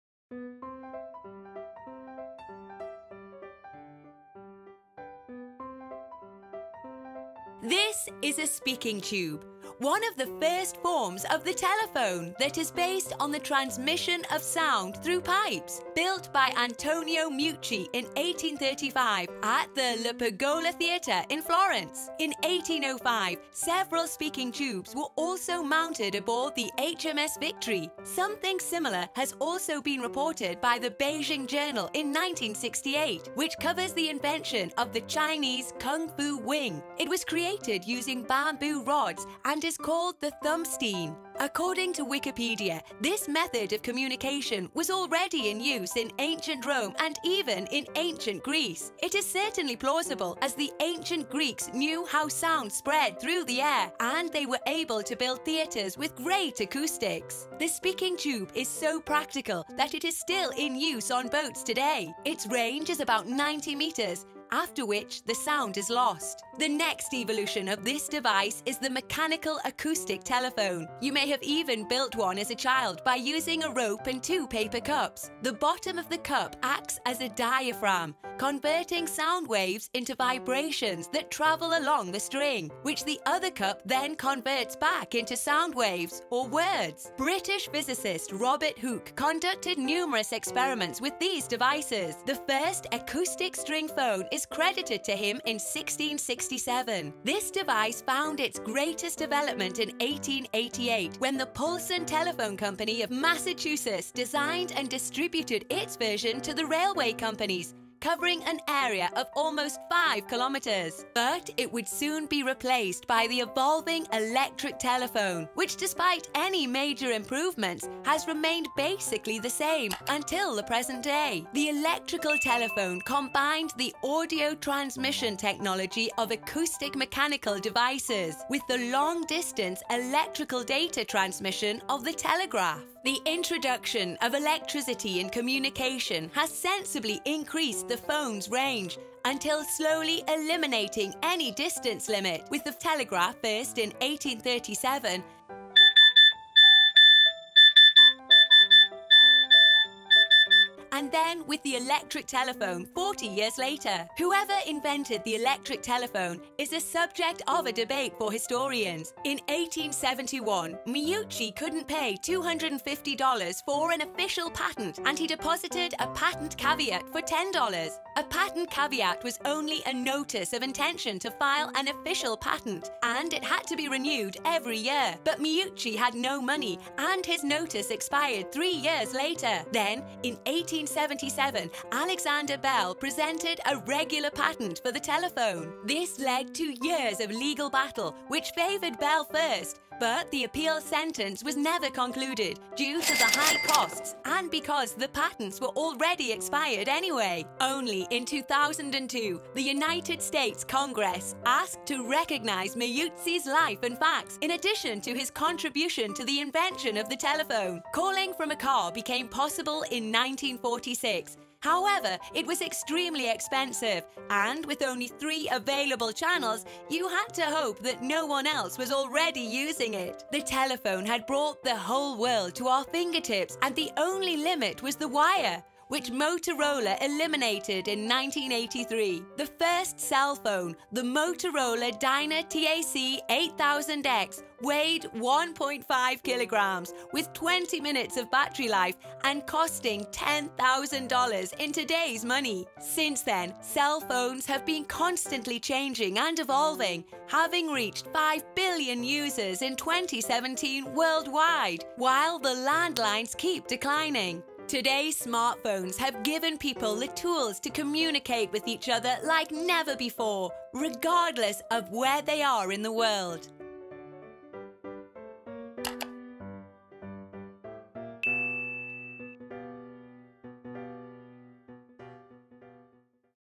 History Of Telephone - From Speaking Tubes To Cell Phones
The history of telephone from the speaking tube Thumstein, the first form of telephone, to the cell phone